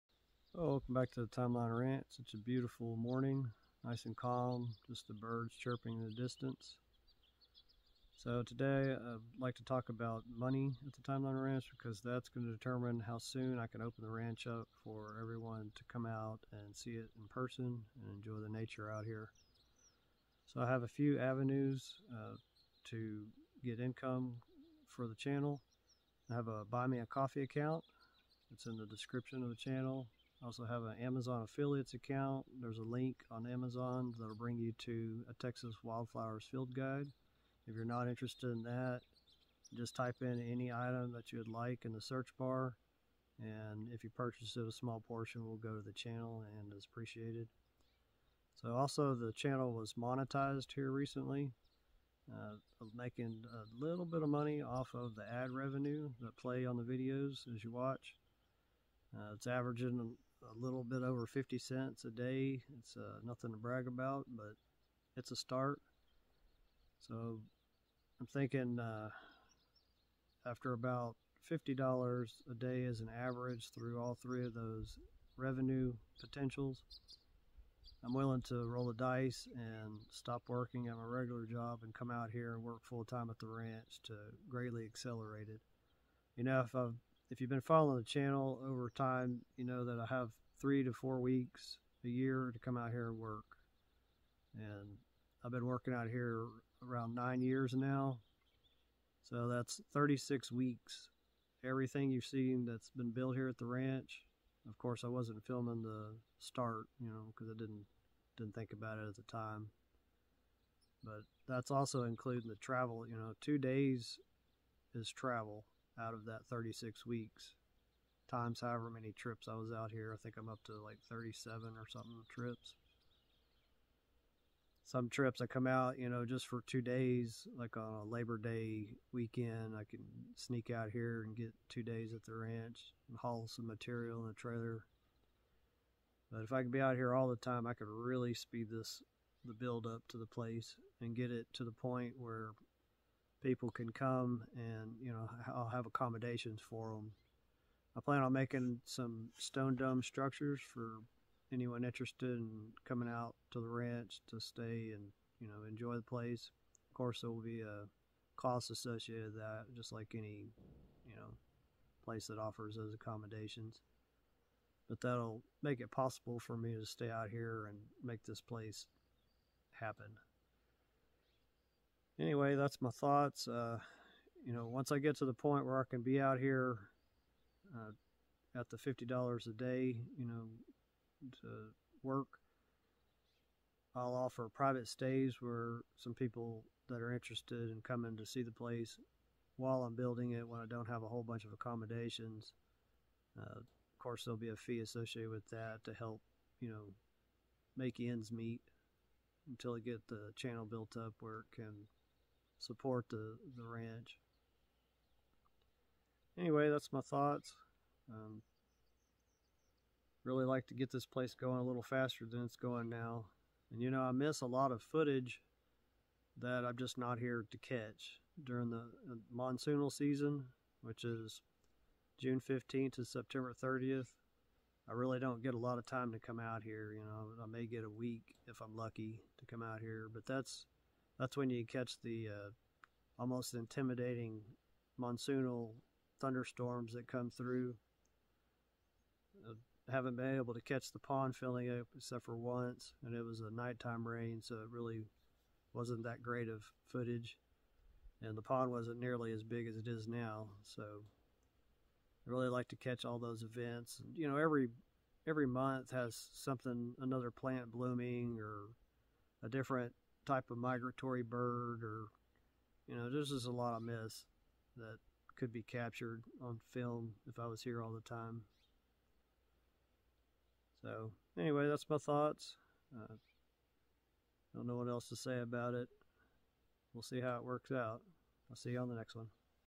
Let's talk money @Timeline Ranch
Chihuahua desert off-grid homestead far west Texas. I'm looking forward to being able to focus on just developing the ranch. The only thing holding me back from making quick progress is time.